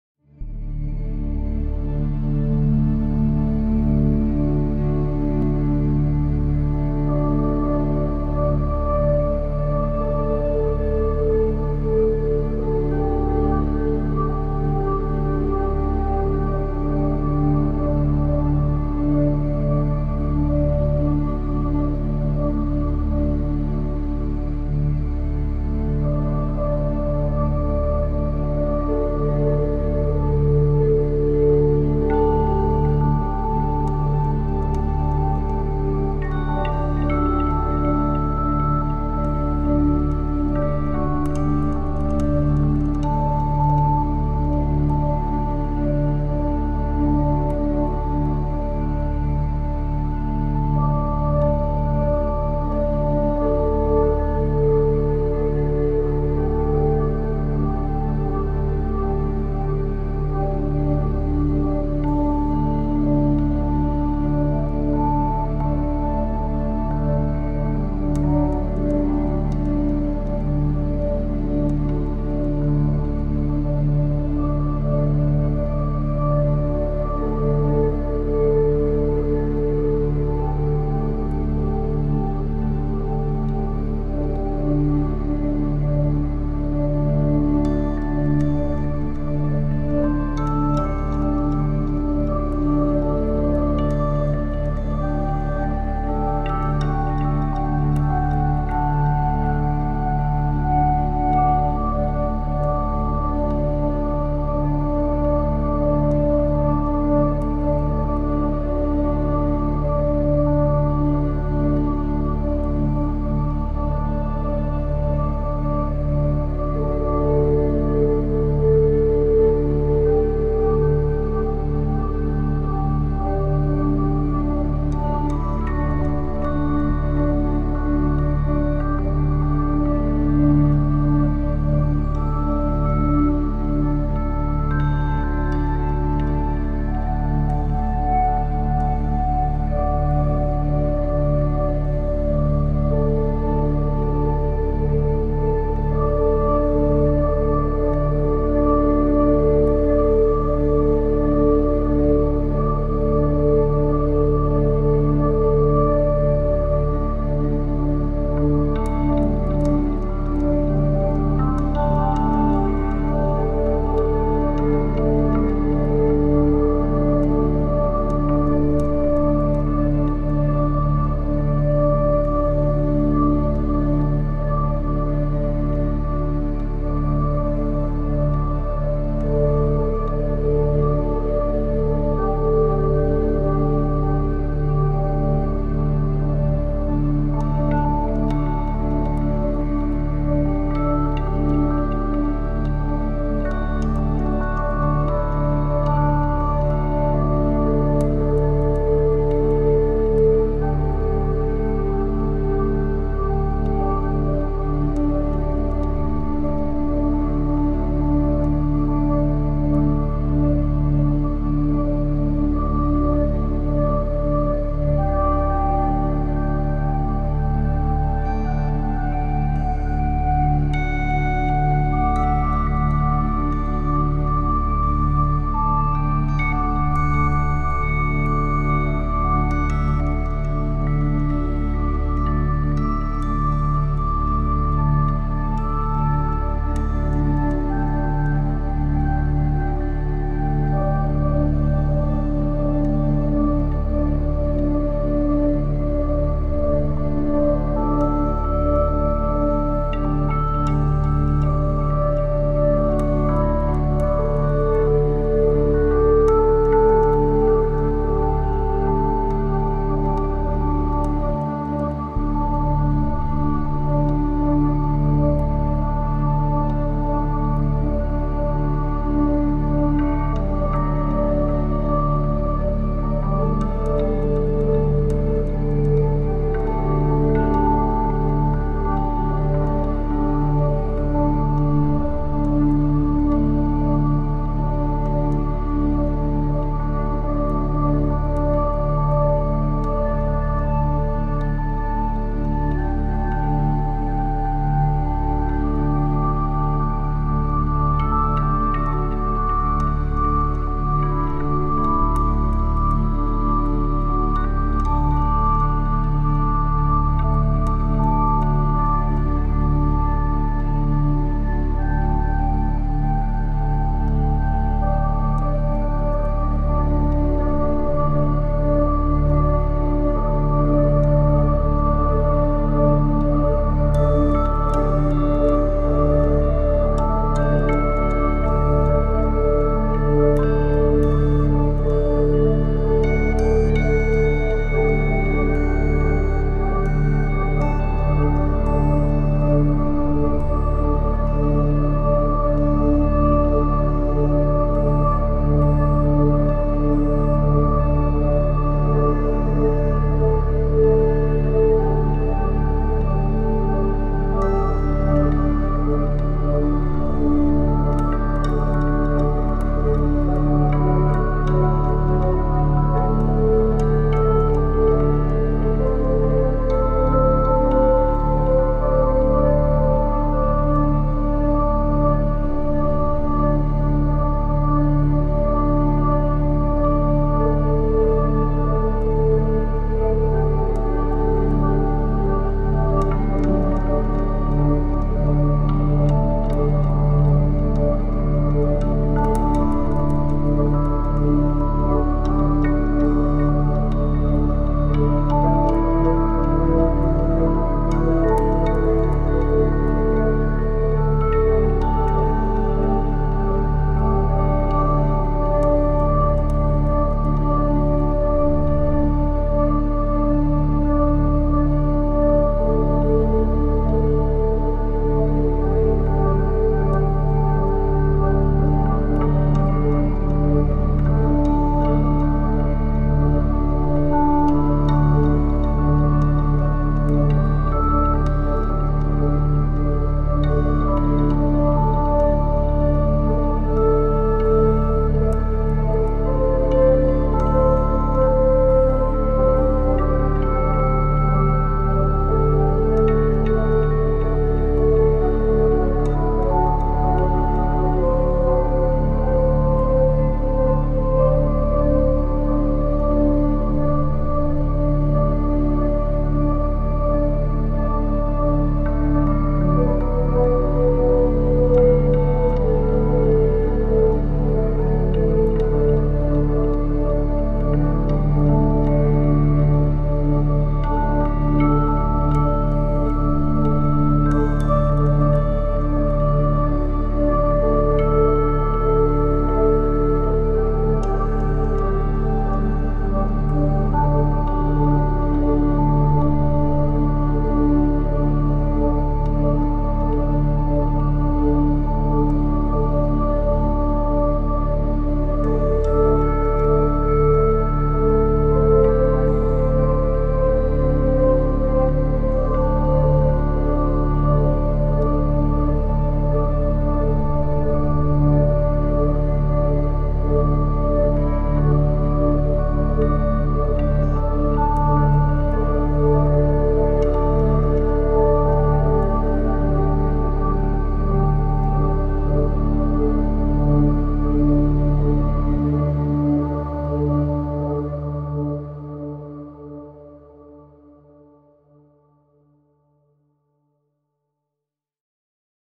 Sacral chakra meditation
Sacral chakra healing,balancing ,meditation music ambient meditation Music:Saara Sutinen